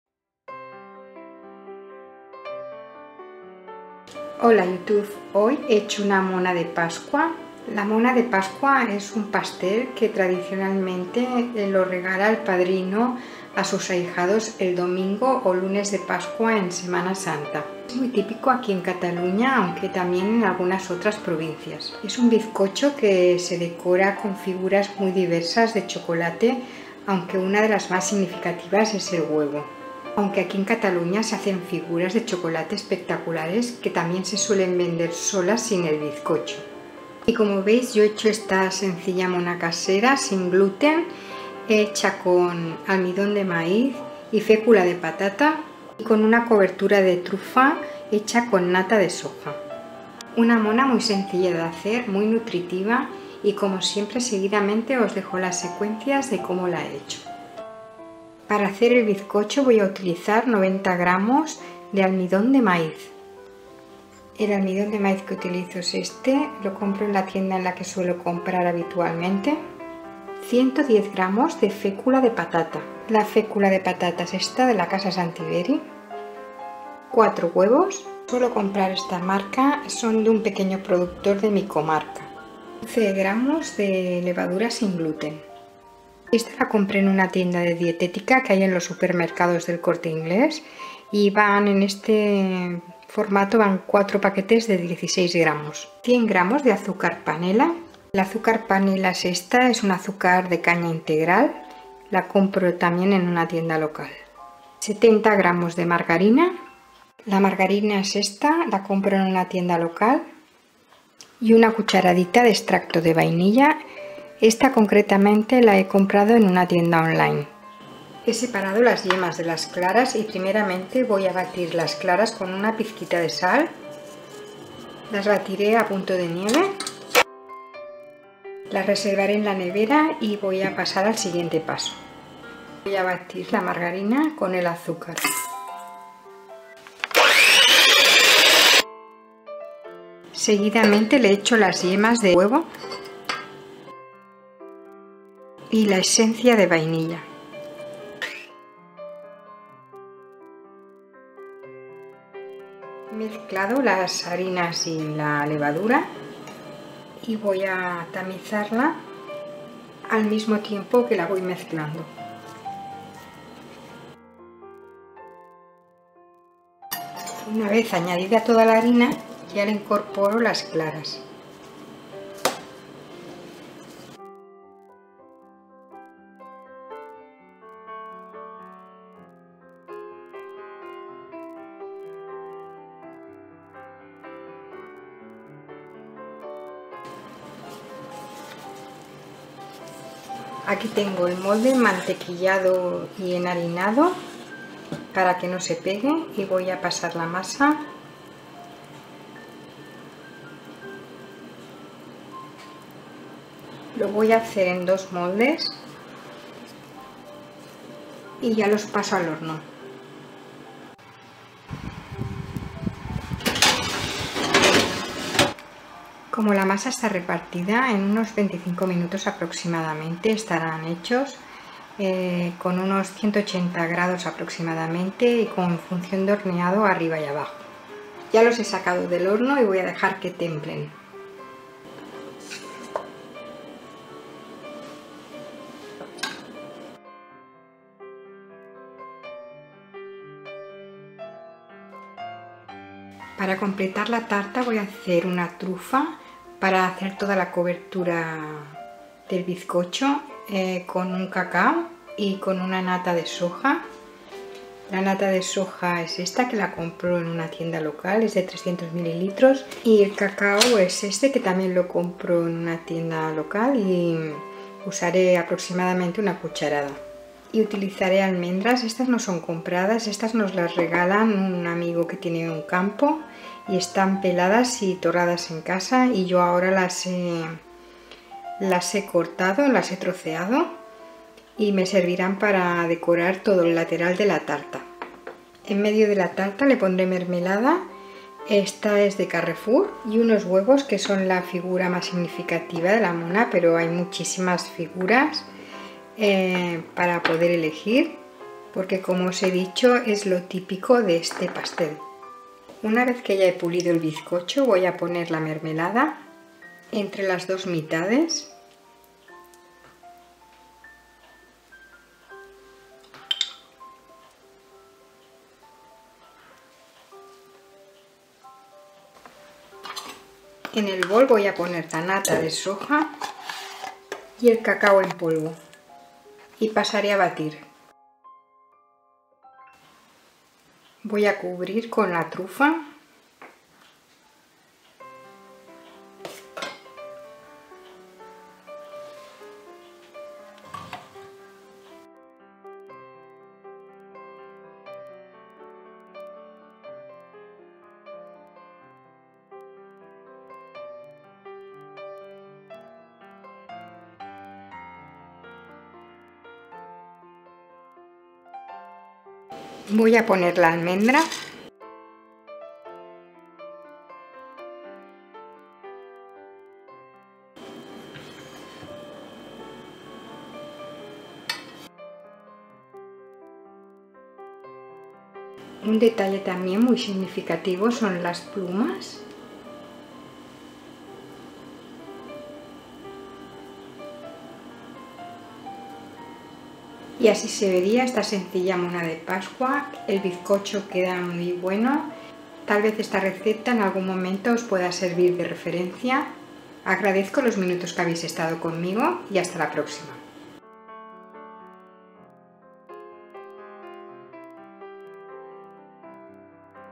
BIZCOCHO , MONA DE PASCUA (Sin Gluten , Sin Lactosa)
Molde 17 cm. Diametro

Ingredientes:
Para el Bizcocho;
90 grs, Almidon de Maiz
110 grs, Fecula de Patata
4 Huevos
11 grs, Levadura sin gluten
100 grs, Azucar de caña integral (Panela)
70 grs, Margarina Vegetal
cucharadita extracto de Vainilla

Para decorar el Bizcocho:
Cacao en polvo ( una cucharada ) al gusto
Nata de Soja (carton de 300 ml.)
Mermelada
Almendras troceadas
Huevos o Figuras de Chocolate